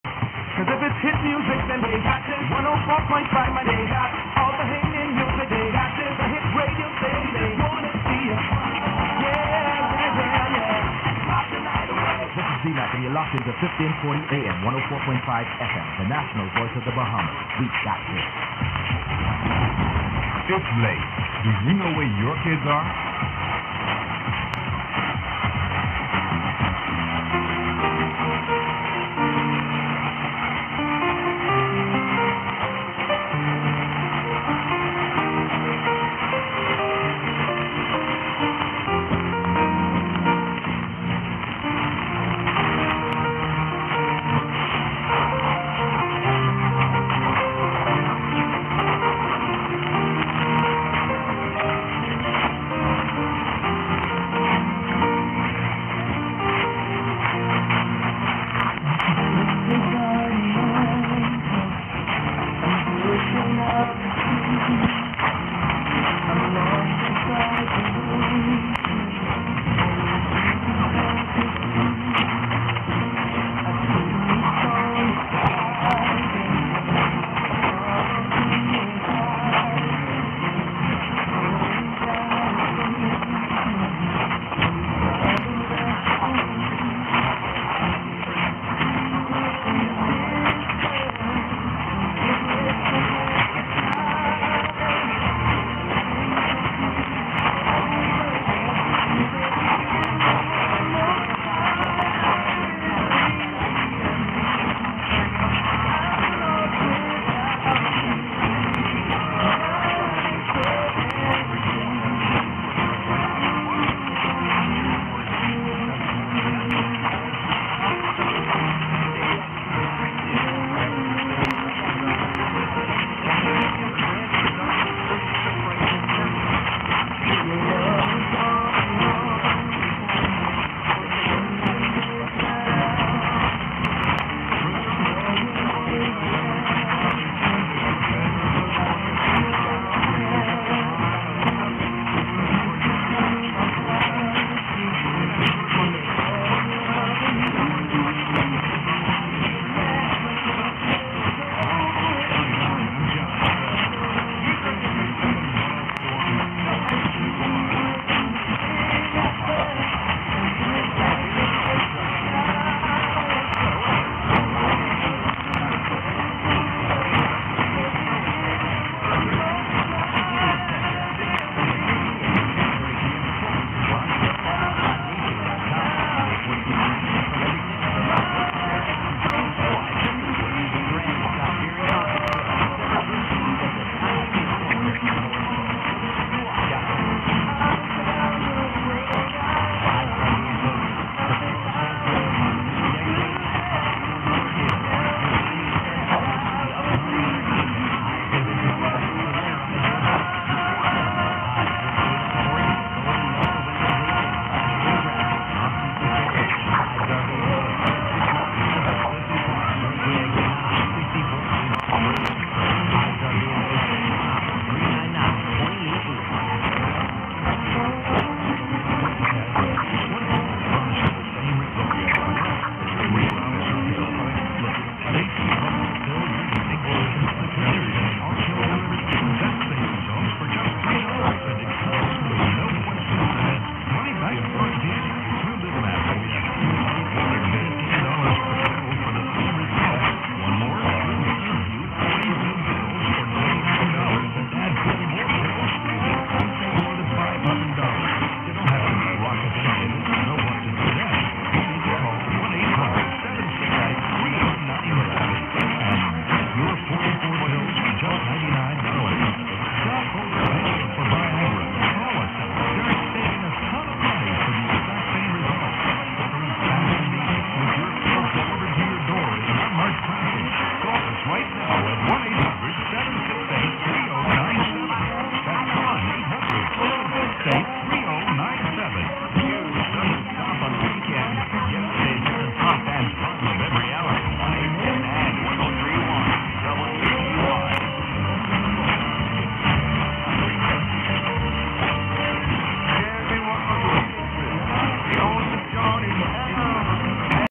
ZNS3 - National Voice of the Bahamas (Bahamas) 810kHz 9/27/15 08:29~UTC - Station Announcement
Received by RadioPerseus in Atlanta, GA, U.S.A. with PERSEUS SDR Receiver & ALA100MS active loop antenna.